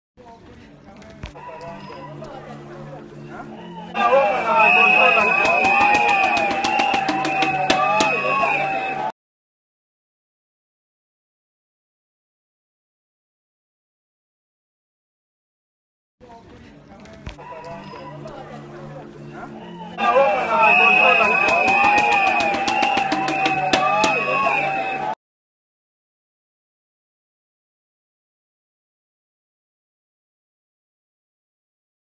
Fires and Protesters on Nairobi Streets Before Election Results Announced
Courtesy of Tyra Angelita Maria